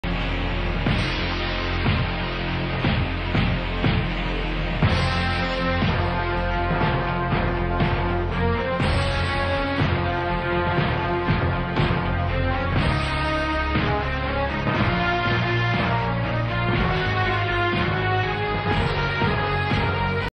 Goldberg spears a baseball player
جولد بيرج يضرب لاعب بيسبول ضربة الرمح